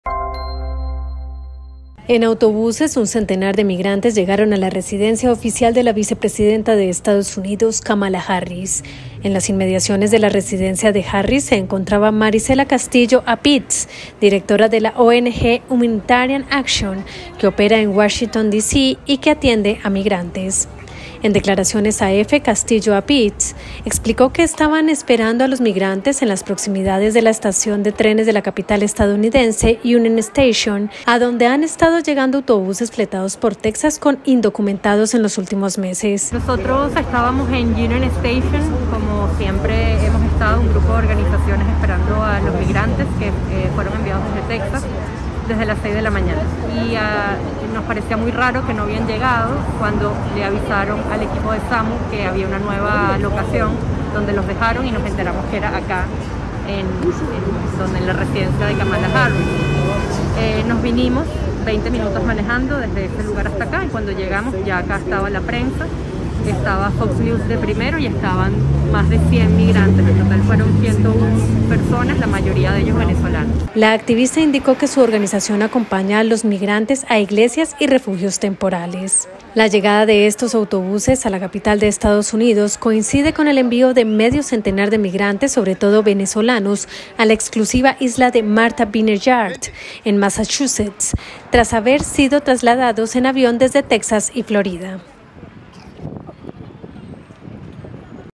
Texas manda buses con centenar de migrantes a residencia de Kamala Harris
Dos autobuses con un centenar de migrantes fletados por el gobernador de Texas, el republicano Greg Abbott, llegaron este jueves a la residencia oficial de la vicepresidenta de Estados Unidos, Kamala Harris.

¡Visítenos!